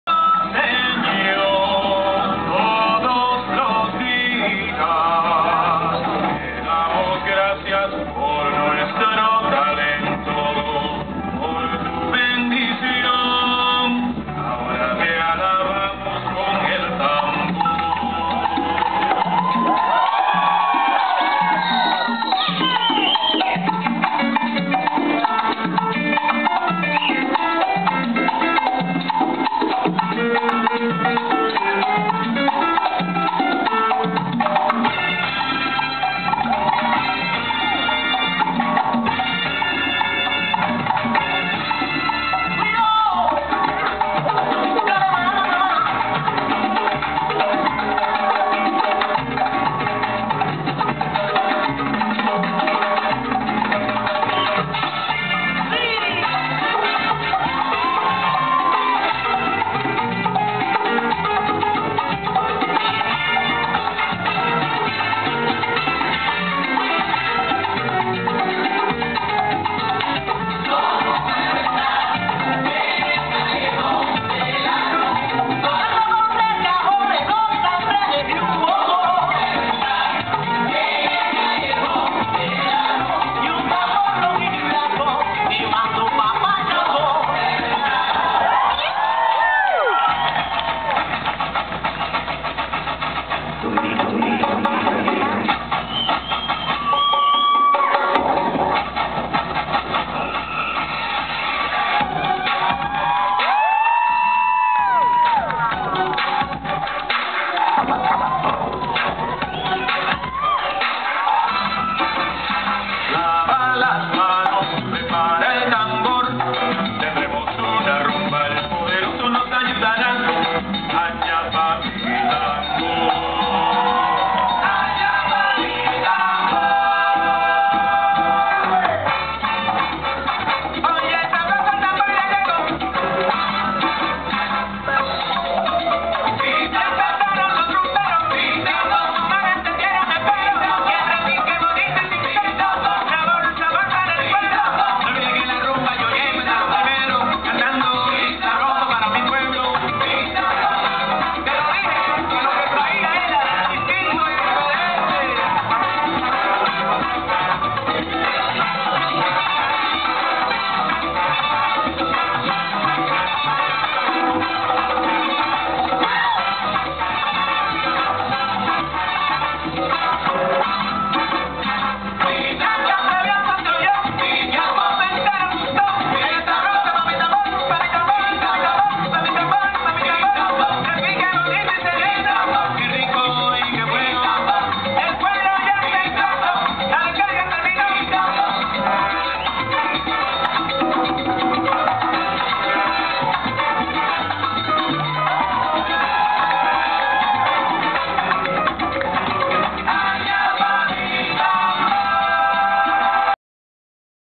SHOW CHIQUITO Y DOMINICAN POWER-CARTHAGO SALSA CONGRESS 2010 (TUNISIA)
LES BONS PLANS DE LA COTE & D'AILLEURS : Click "I like/J'aime" on our
SHOW CHIQUITO Y DOMINICAN POWER (Dominican Republic)
SAM 09 OCT, 2010 (Hotel Caribbean World Carthage)